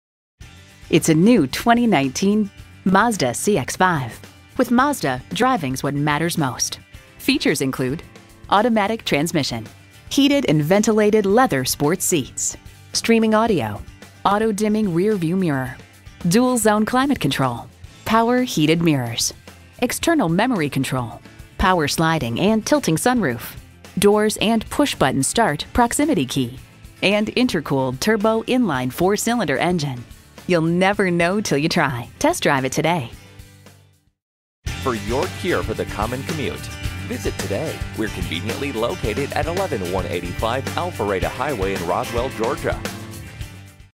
New 2019 Mazda CX-5 Roswell GA Atlanta, GA #610382
Year: 2019
Make: Mazda
Model: CX-5
Trim: Signature
Engine: 2.5L 4-Cylinder Turbocharged
Transmission: 6-Speed Automatic
Color: Black
Interior: Caturra Brown
Stock #: 610382
VIN: JM3KFBEY4K0610382
 Recent Arrival! $1,518 off MSRP! Jet Black Mica 2019 Mazda CX-5 Signature AWD 6-Speed Automatic 2.5L 4-Cylinder Turbochargedbrbr22/27 City/Highway MPG

Address:
11185 Alpharetta Hwy